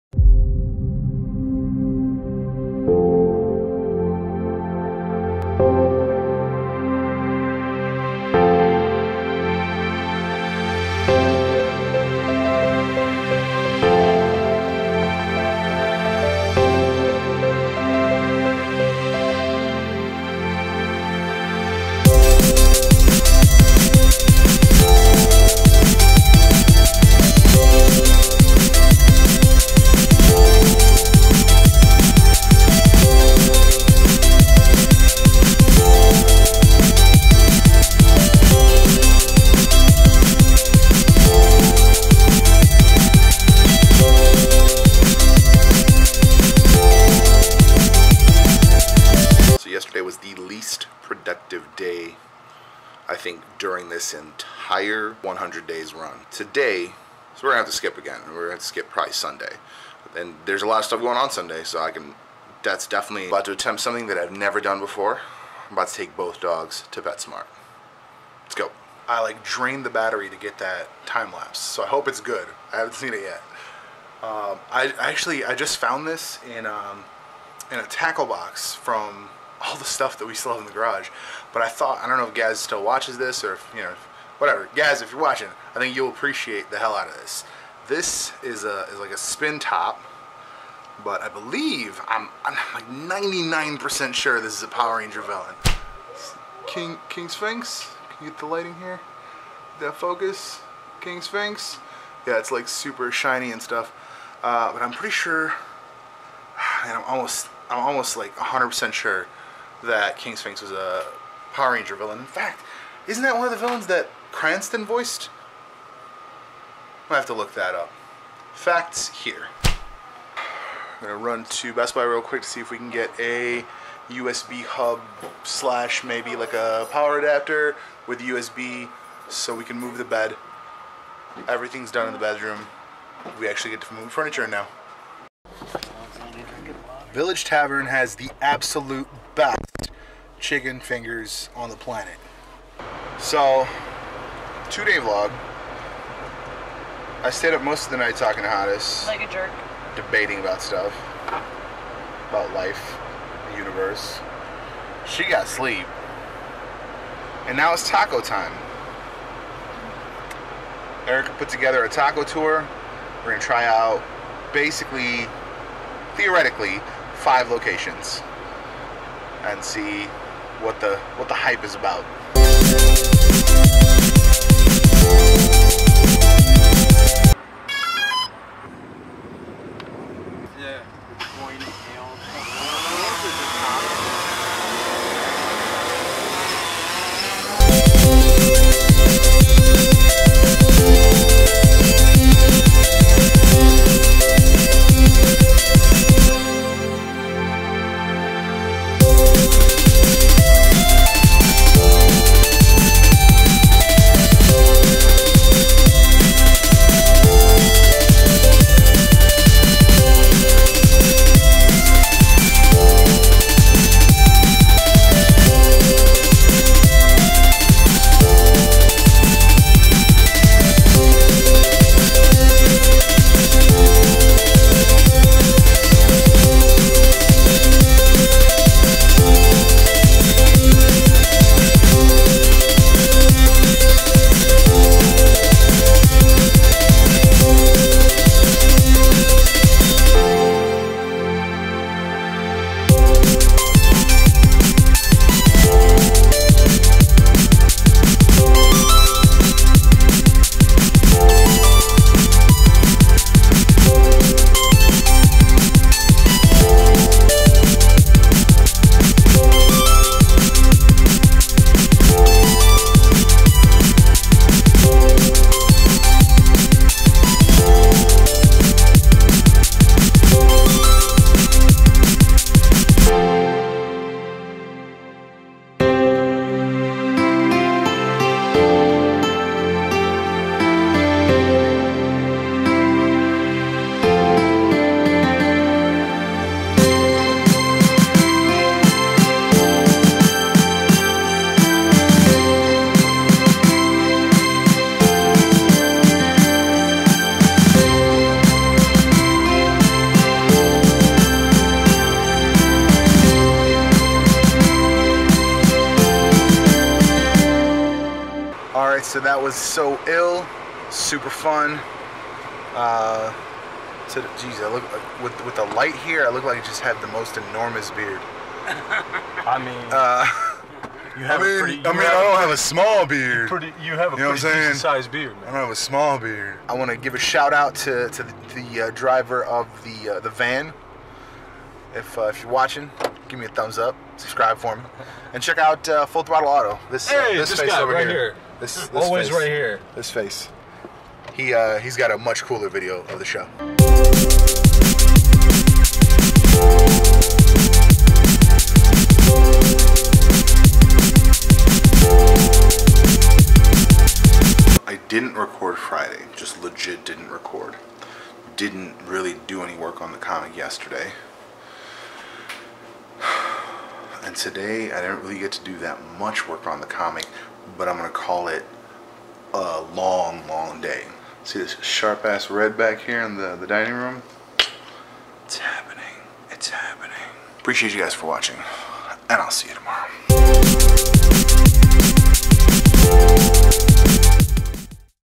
Vlog 107 - NEXUS 100 Day 71
So Ill car show, chicken fingers and more!

T3G Social:

Beme: Those3Guys